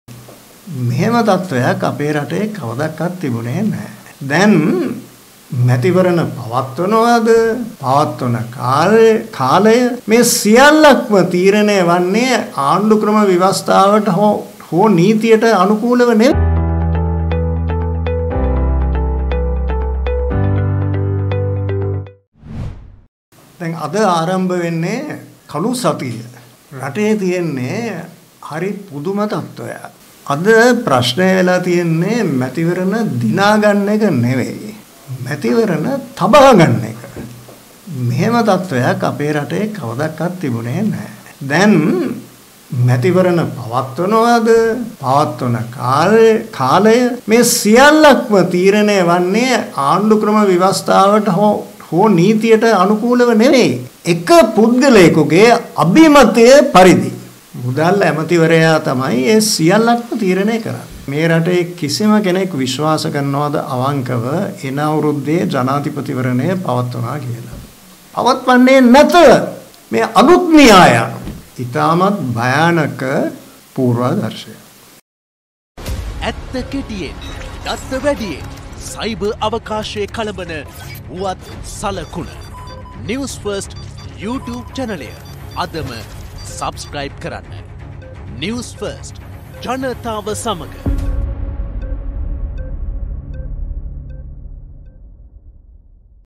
අද ප්‍රශ්නය වෙලා තියෙන්නේ මැතිවරණ දිනා ගන්න එක නෙමෙයි
නිදහස් ජනතා සන්ධානය අද කොළඹදී කැදවා තිබූ මාධ්‍ය හමුවකදී පළාත් පලාන මැතිවරණය සම්බන්ධයෙන් අදහස් පල කලා